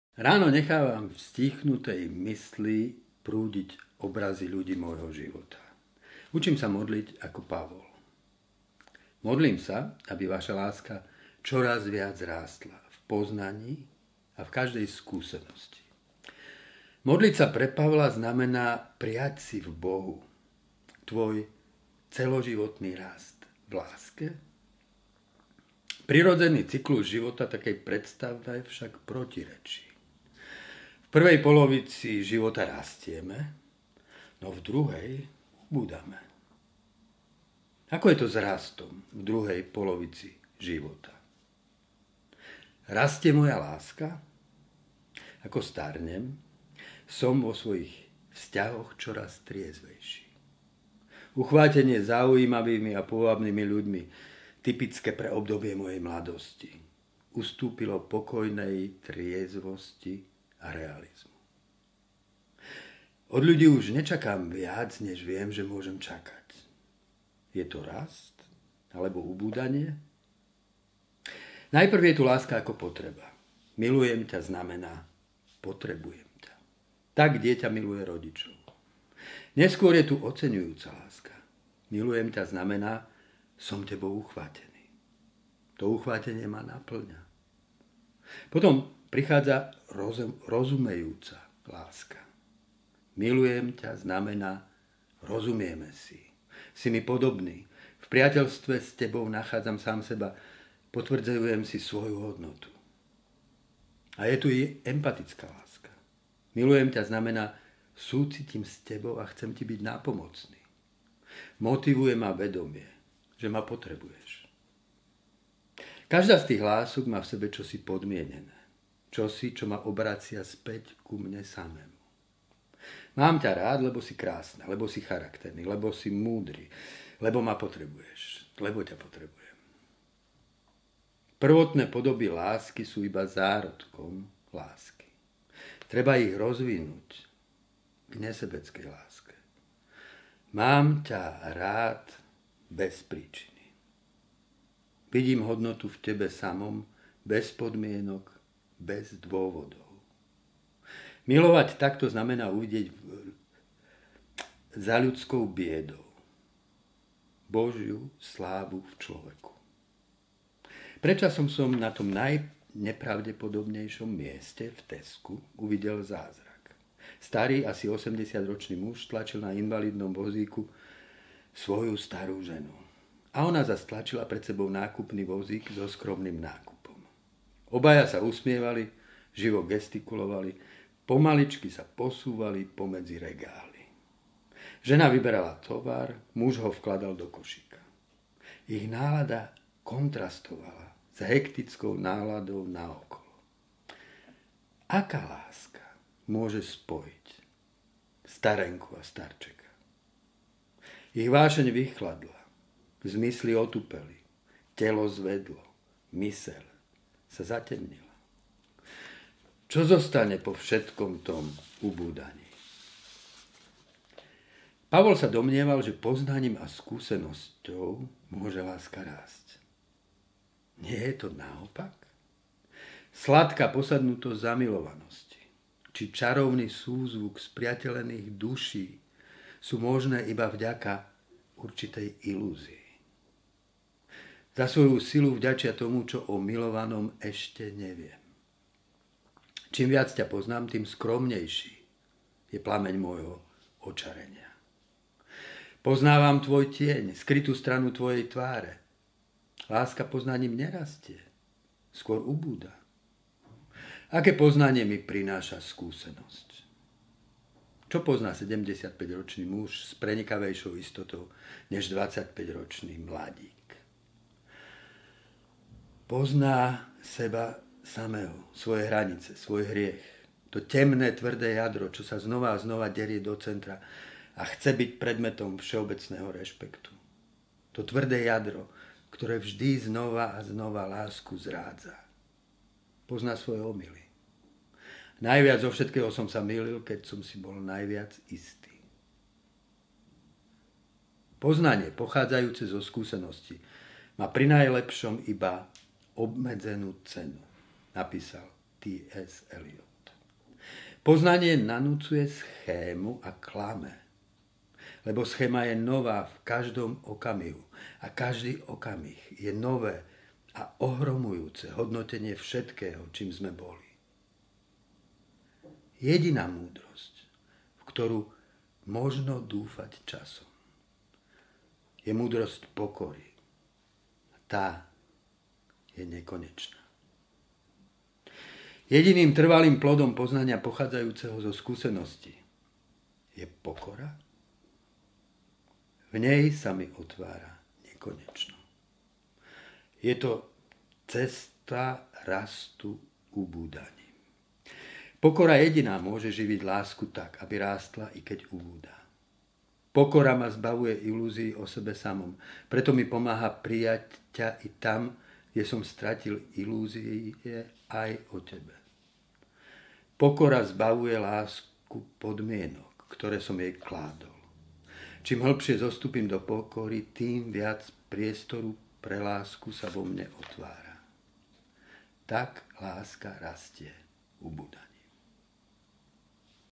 K rannej káve #17 - Môže láska rásť?
Každý utorok a štvrtok o 8:00 pár slov k zamysleniu a meditácii
Slovo: Daniel Pastirčák